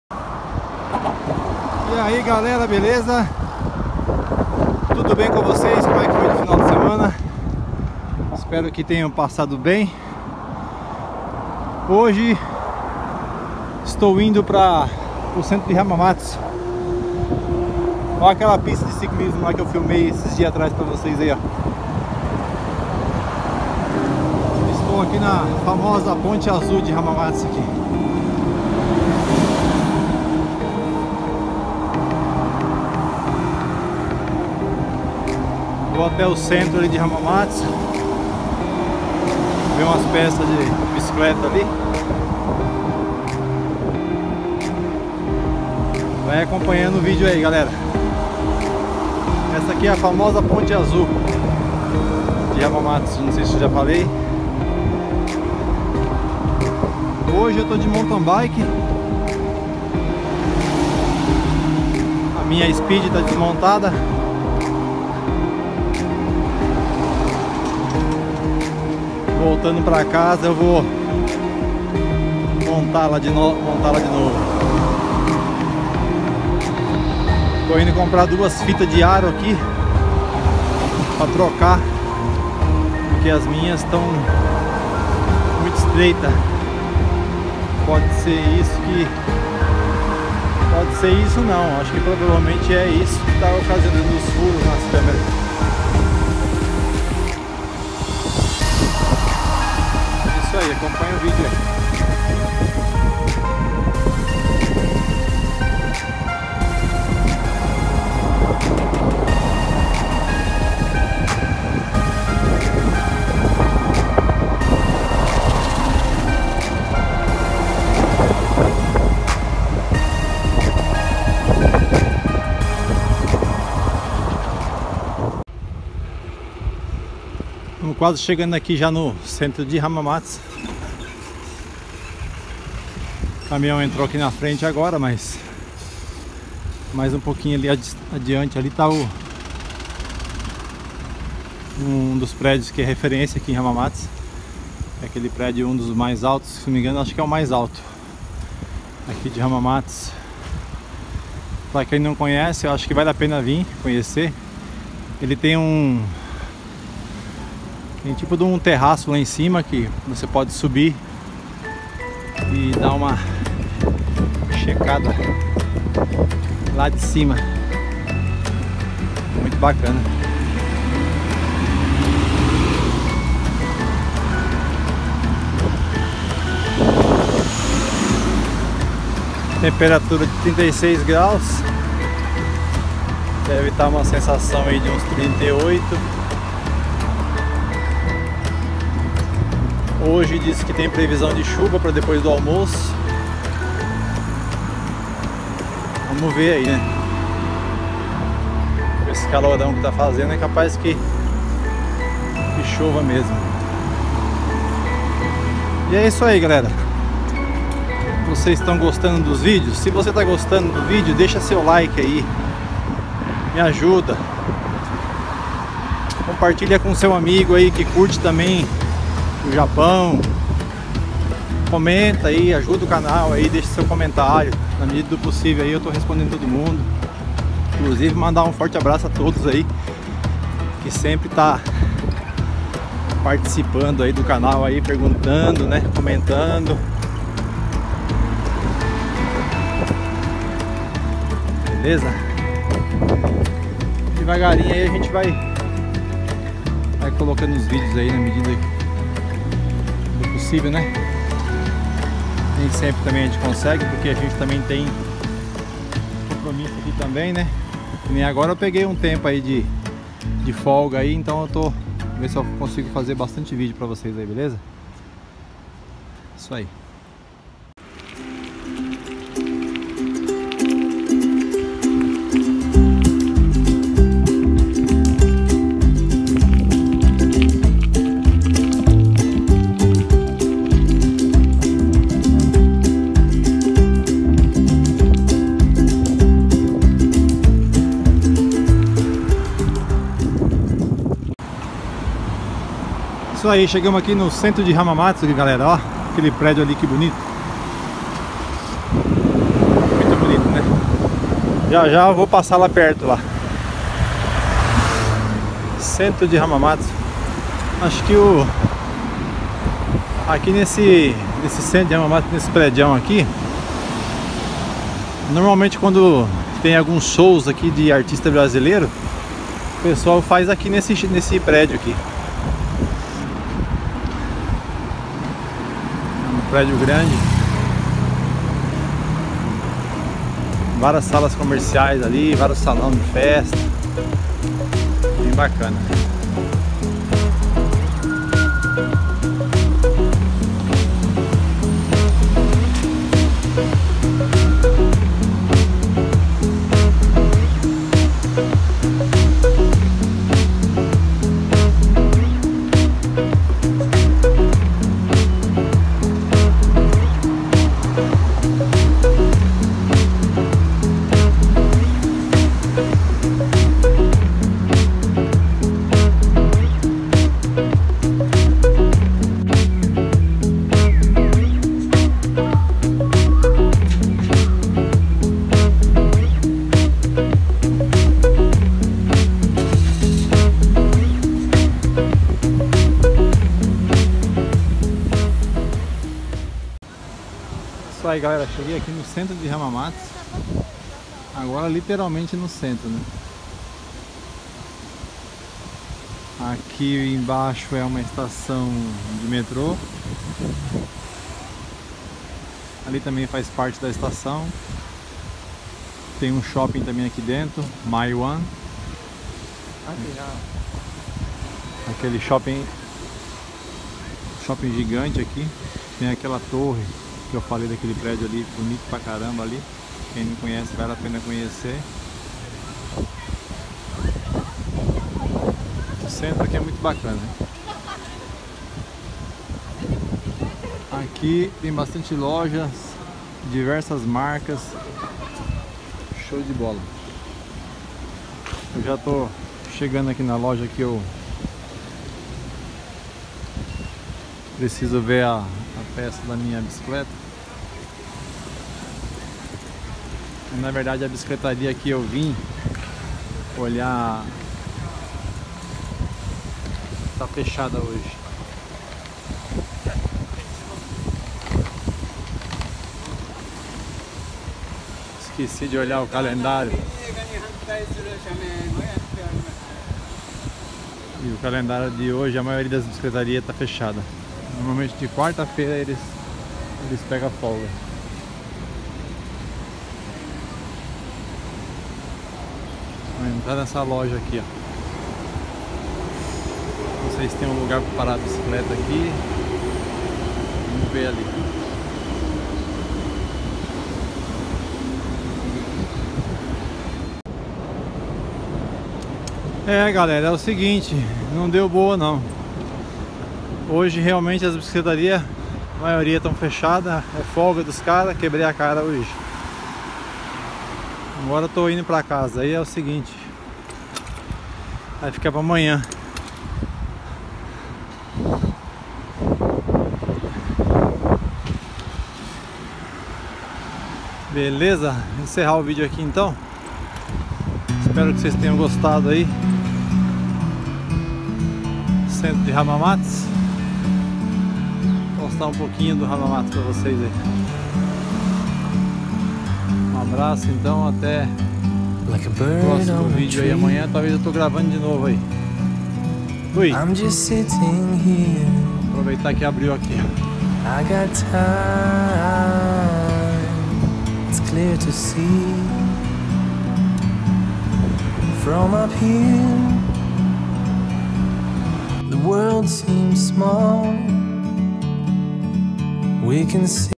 A Caminho de Hamamatsu
Hamamatsu é uma cidade japonesa localizada na província de Shizuoka. Em 1 de março de 2018, a cidade tinha uma população estimada em 806.435 habitantes, sendo cerca de 12 mil brasileiros, fazendo com que a cidade tenha a maior concentração de brasileiros per capita do Japão.

Me siga também no Instagram : Pedal no japao, Facebook ; Marcelo Japão.